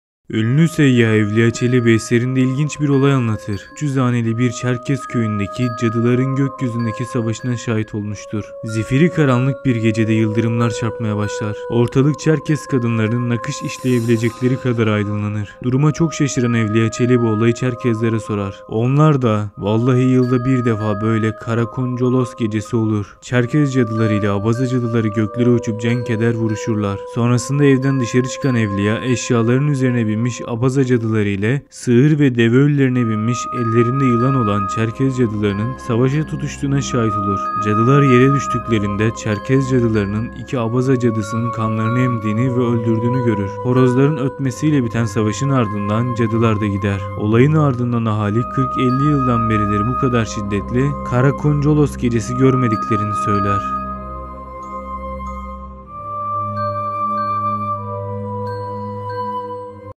OSMANLI'DA ÇERKES CADILARI NEDEN SAVAŞIYORDU?
Evliya Çelebi'nin anlattığı ilginç hatıra